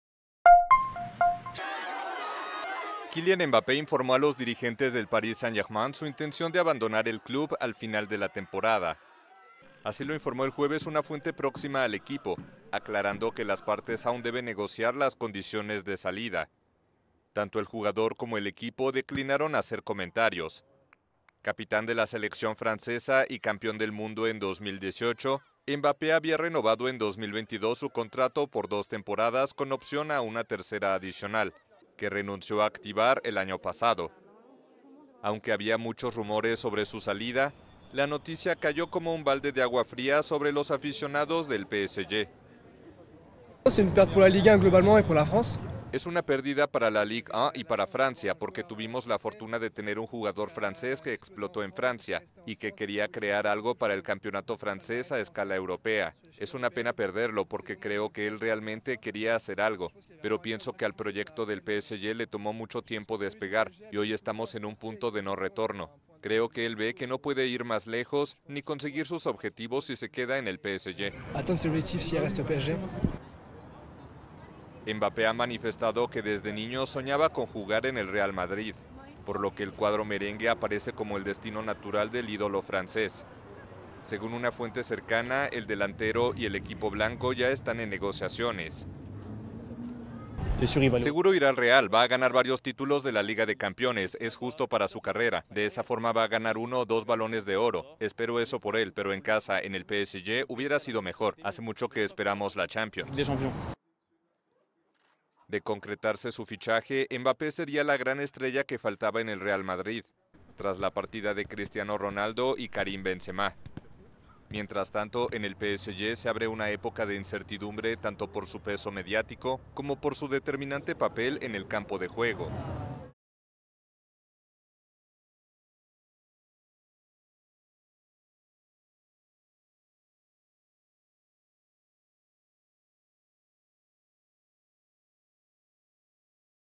Mbappé se va del PSG
El delantero Kylian Mbappé ha anunciado a los dirigentes del París Saint-Germain su intención de abandonar el club.